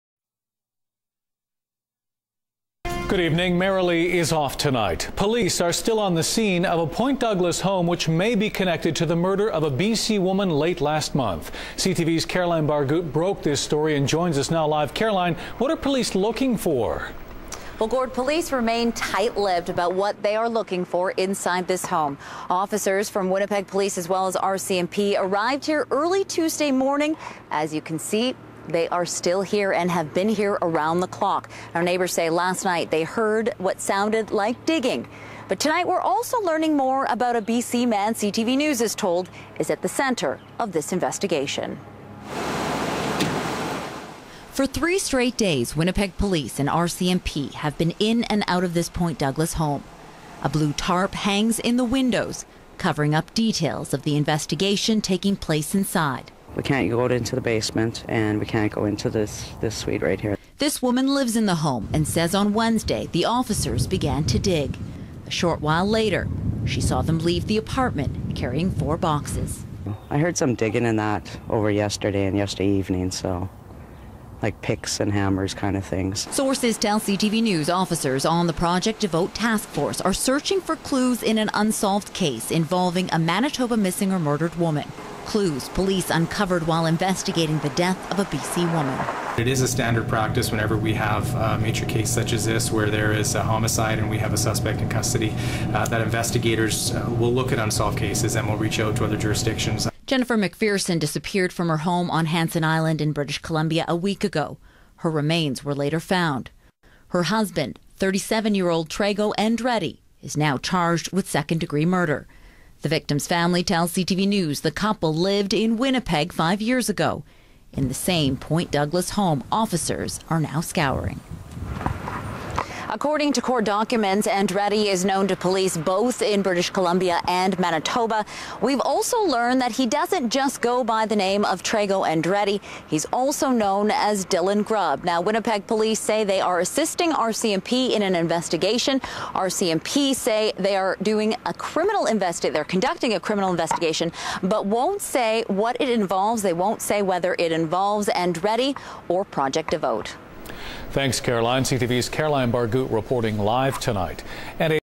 cba murder
This is a follow up to the story I broke about the search for a missing woman believed buried in the basement of this home.